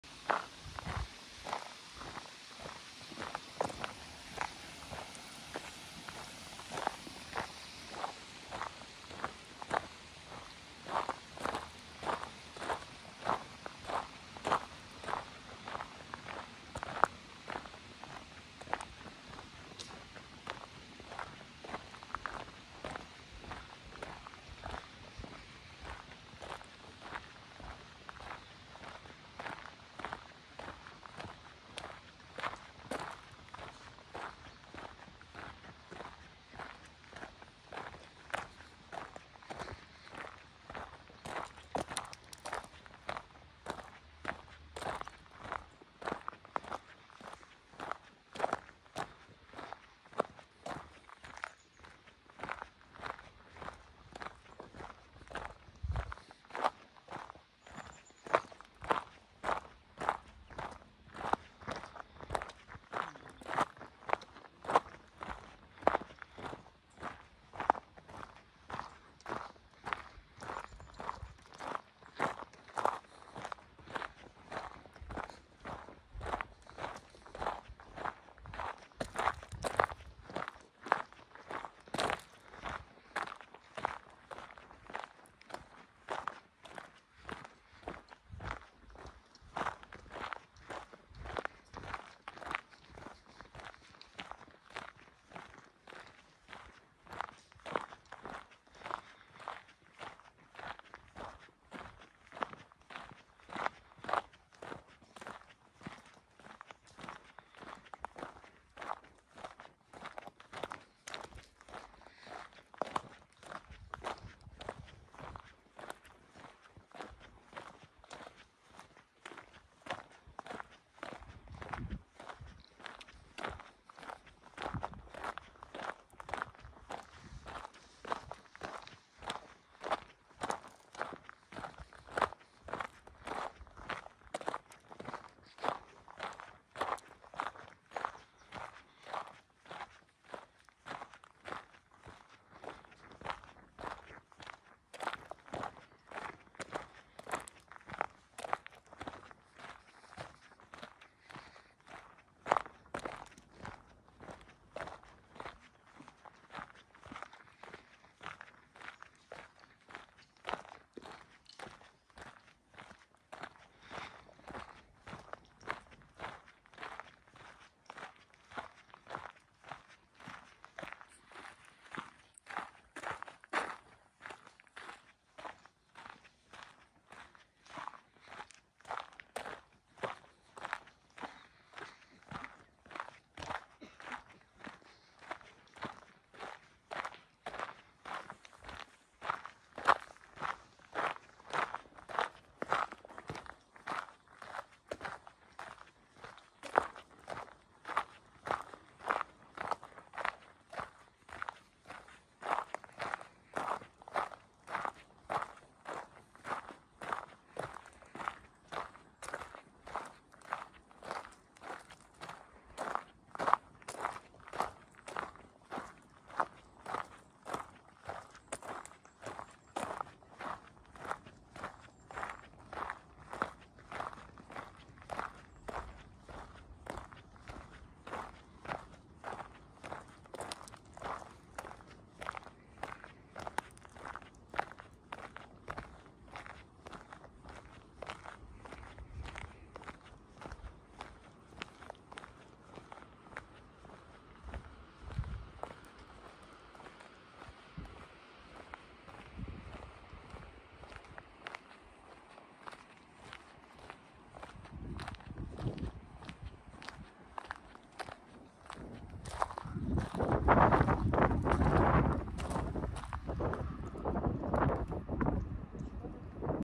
Hiking in the Swiss Alps (Part 8)
In this video, we continue our hiking trip in the Swiss Alps.

In part 2 of our hiking trip in the Swiss Alps, we explore more mountainous terrain . This is a great hike for anyone looking for a scenic hike in the Swiss Alps!